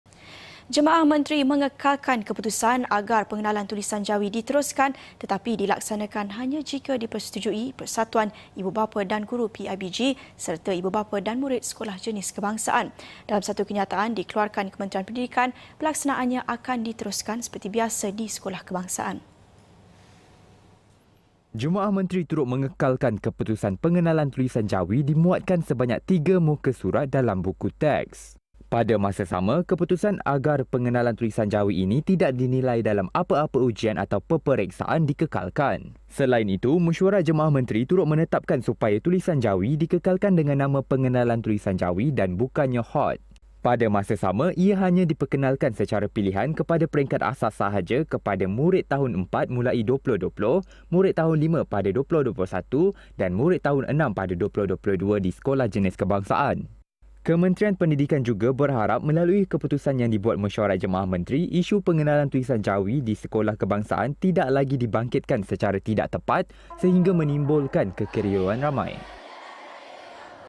Pengenalan Jawi diterusakan jika dipersetujui PIBG, dan murid SJK
Menteri Pembangunan Usahawan, Datuk Seri Mohd Redzuan Yusof menyifatkan kenyataan Perdana Menteri, Tun Dr Mahathir Mohamad Persatuan Gabungan Jawatankuasa Sekolah Cina (Dong Zong) sebagai rasis adalah tidak salah.
Tegasnya, mana-mana pihak yang tidak mahu bersama-sama memperjuangkan asas perlembagaan boleh dikategorikan sedemikian.